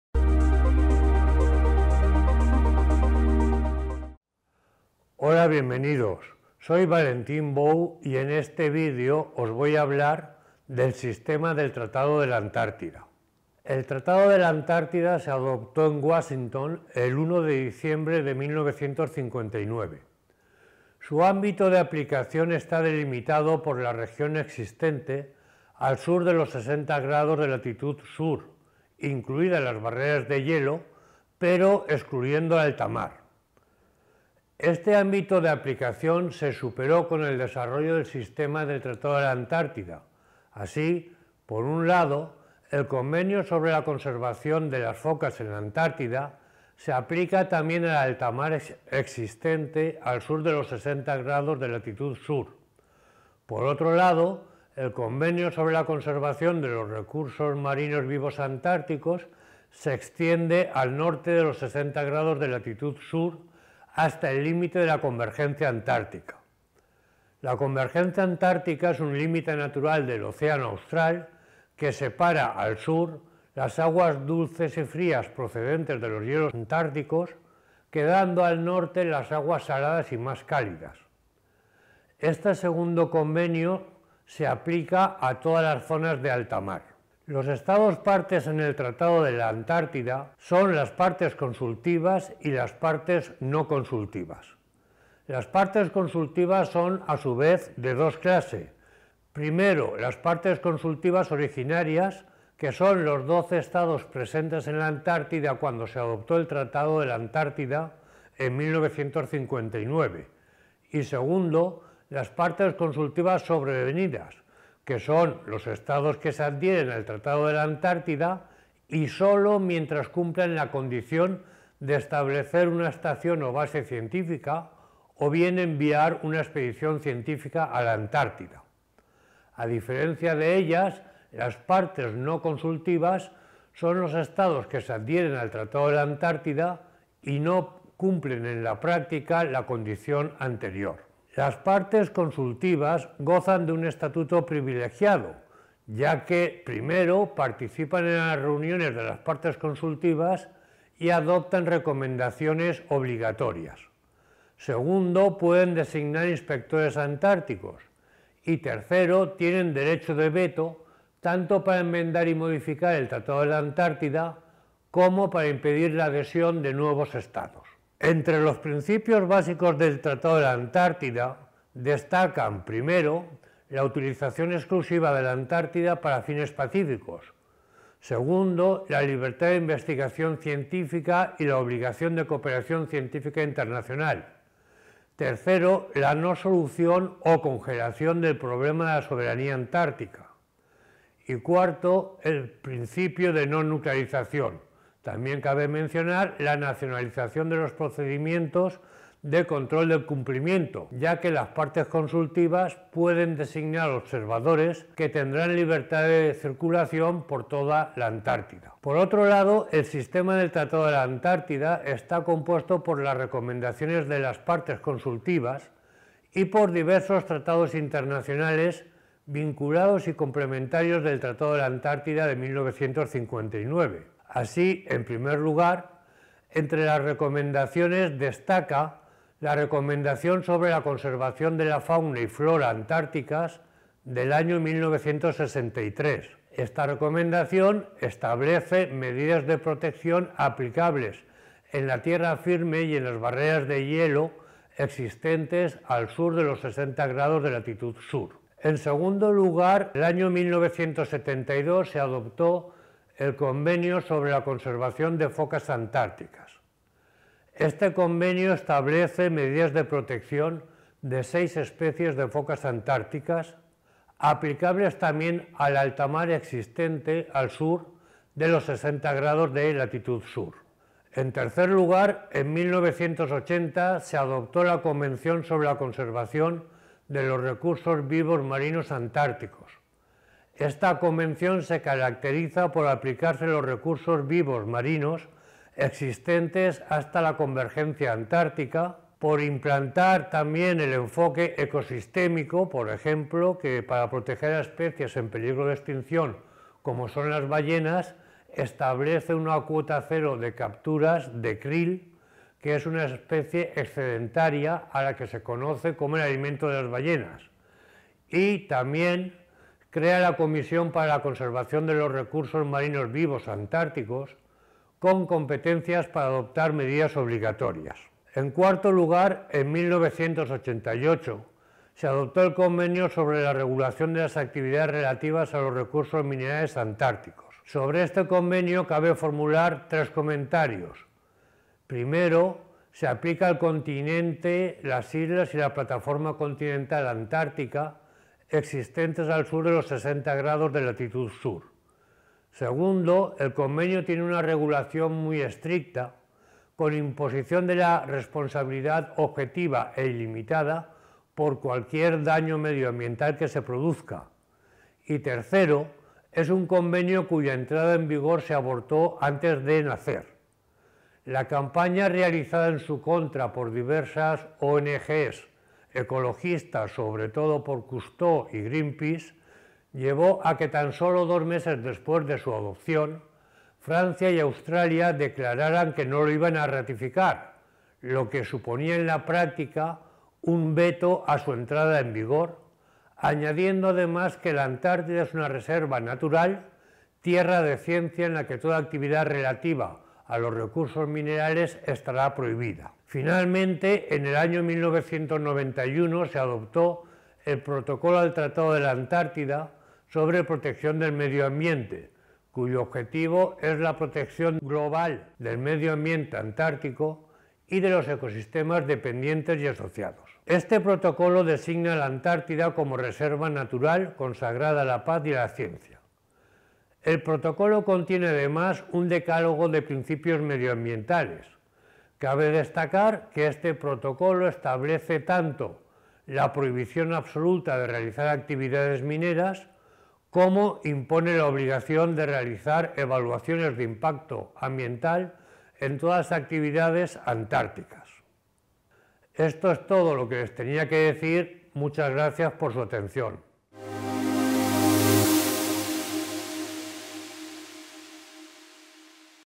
El sistema del Tratado de la Antártida
Autor: Valentín Bou Franch

En este vídeo explico brevemente el Tratado de la Antártida, así como los instrumentos jurídicos más relevantes que conforman el sistema del Tratado de la Antártida.